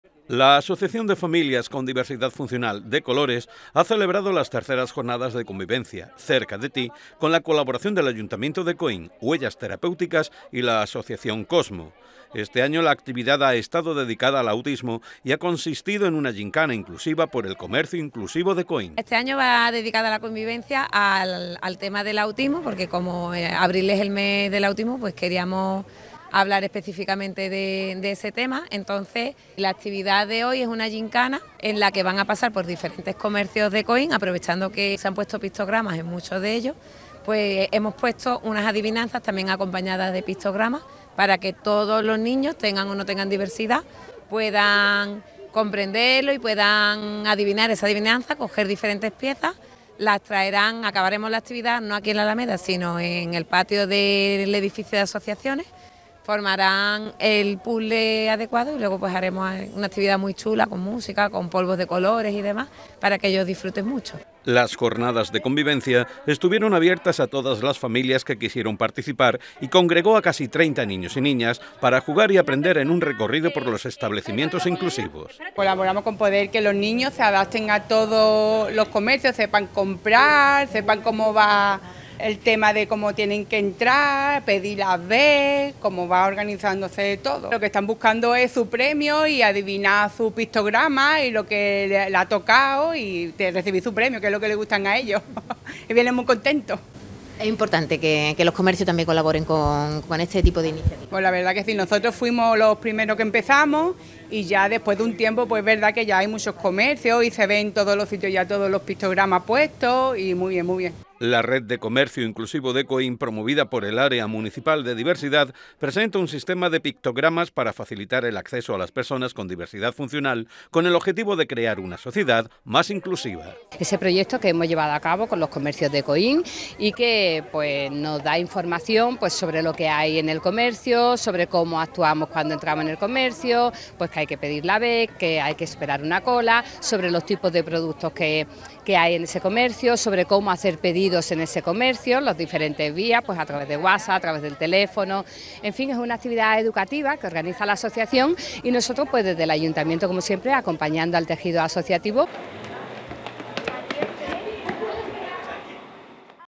III JORNADAS COINVIVENCIA DECOLORES
La asociación de familias con diversidad funcional DeColores ha celebrado las terceras jornadas de convivencia ‘Cerca de ti’, con la colaboración del Ayuntamiento de Coín, Huellas Terapéuticas y la asociación Khosmo. Este año la actividad ha estado dedicada al autismo y ha consistido en una gymkhana inclusiva por el comercio inclusivo de Coín.
Las jornadas de convivencia estuvieron abiertas a todas las familias que quisieron participar y congregó a casi treinta niños y niñas para jugar y aprender en un recorrido por los establecimientos inclusivos.
La red de comercio inclusivo de Coín promovida por el área municipal de Diversidad, presenta un sistema de pictogramas para facilitar el acceso a las personas con diversidad funcional con el objetivo de crear una sociedad más inclusiva.